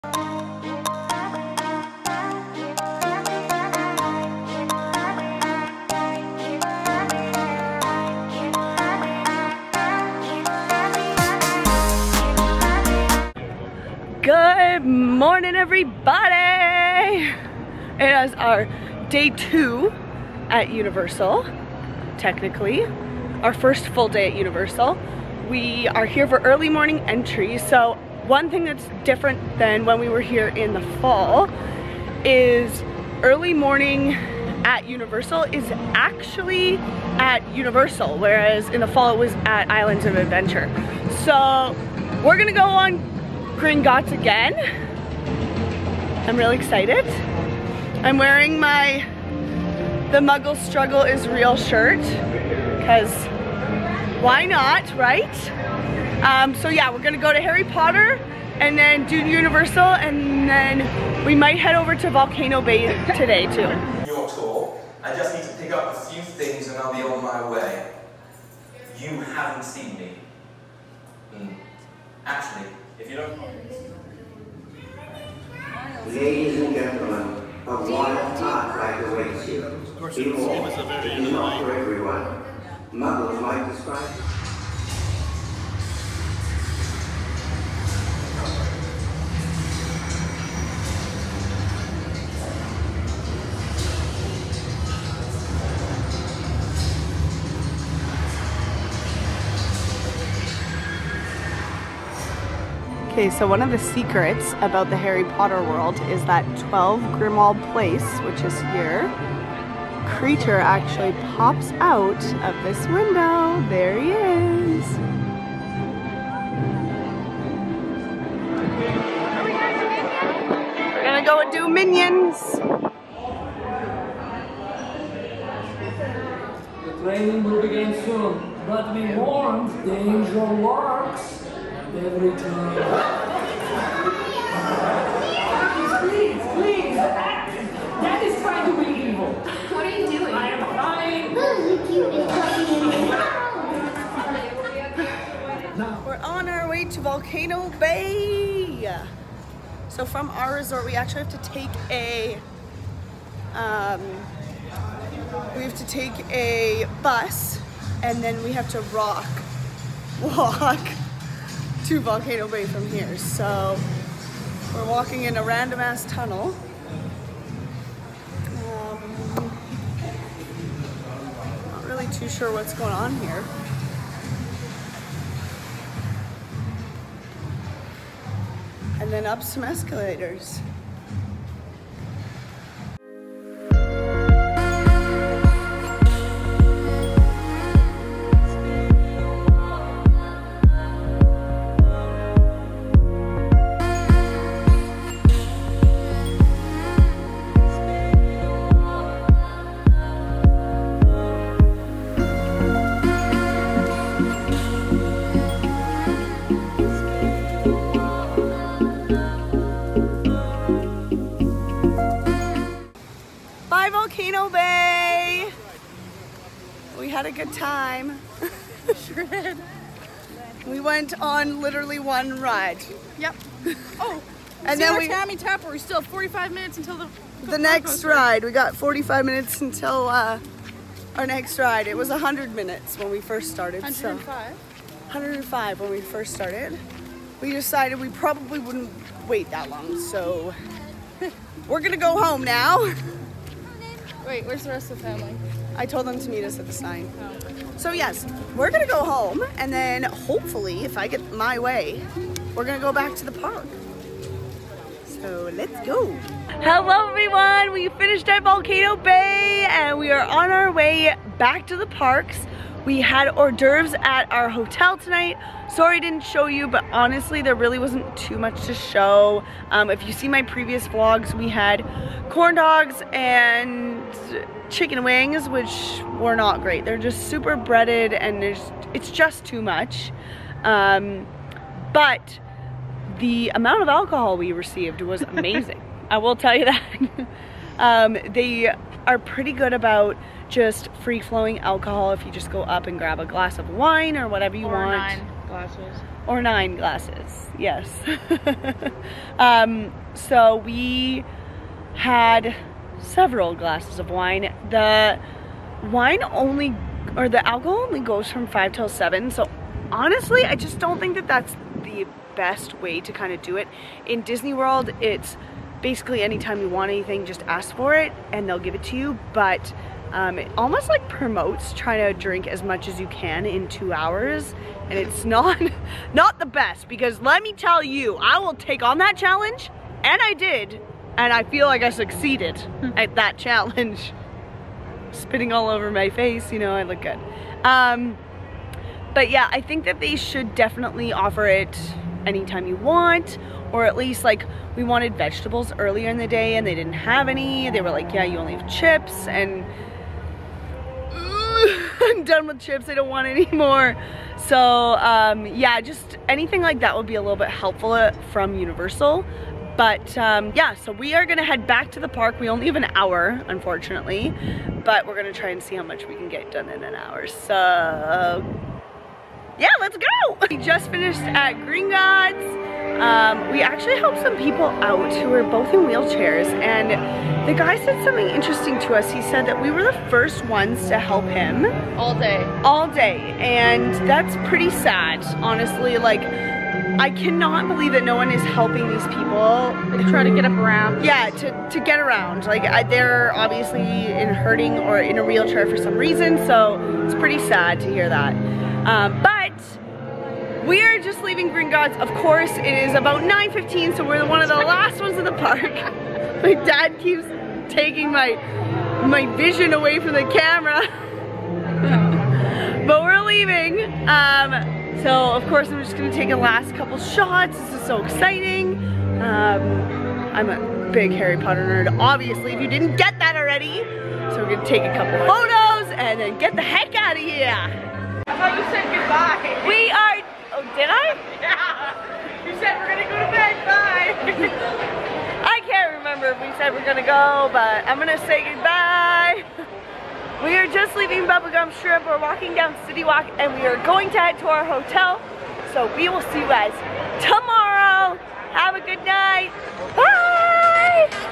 Day 7 | Slow down and help those in need
Day 7 has arrived and we have decided to hit up Volcano Bay for the first time! After only a few hours we decided to leave due to the long lines. Then we head back to the parks where we find out the truth behind people who have a disability. Enjoy!